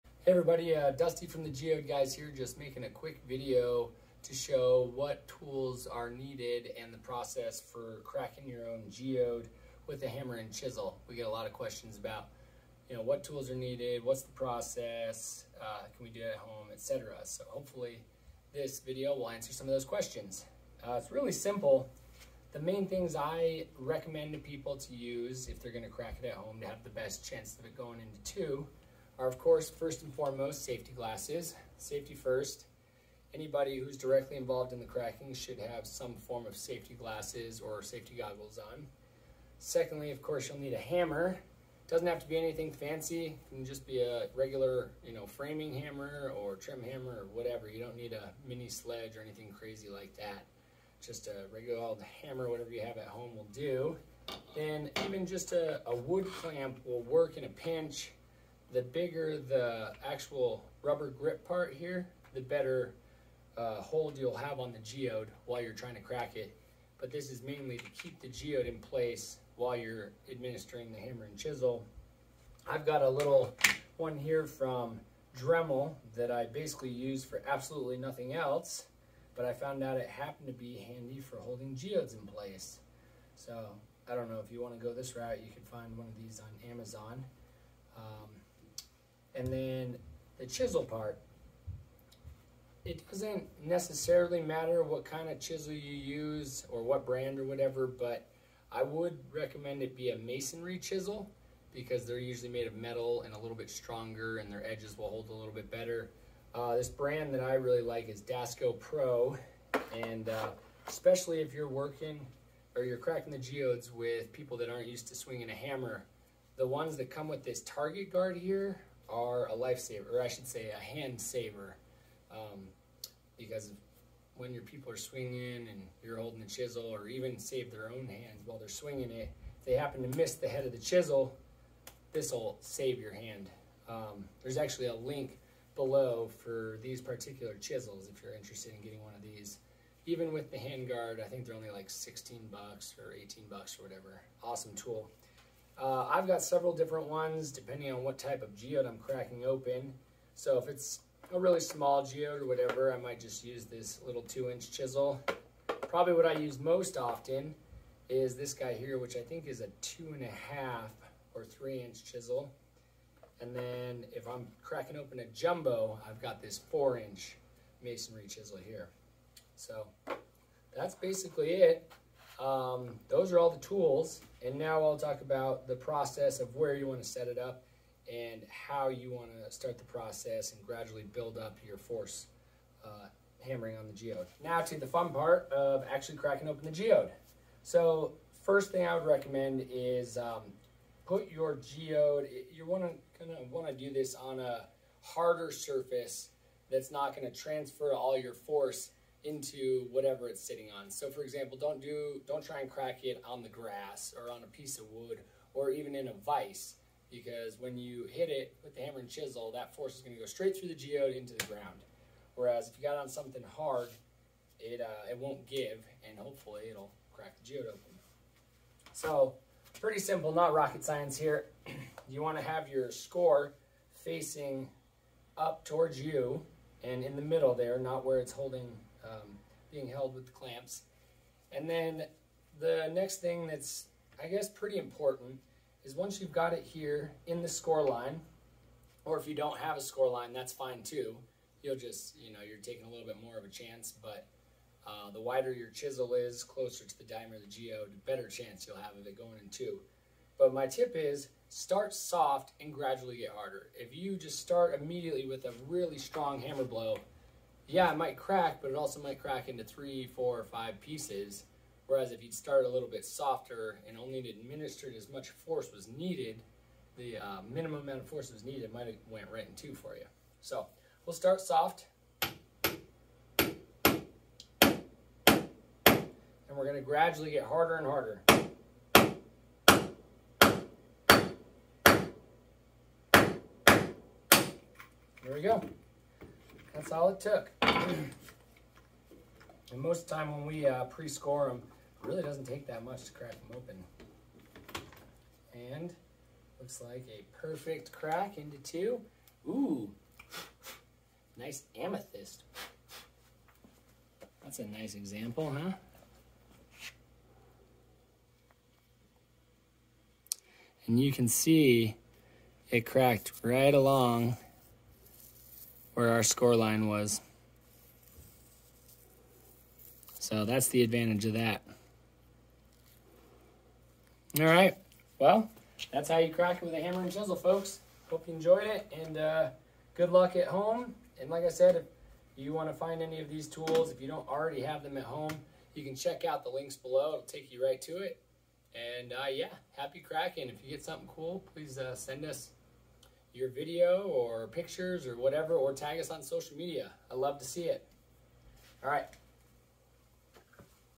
How to crack open a Geode tutorial
How to crack a Geode with a hammer and chisel.

Dremel clamp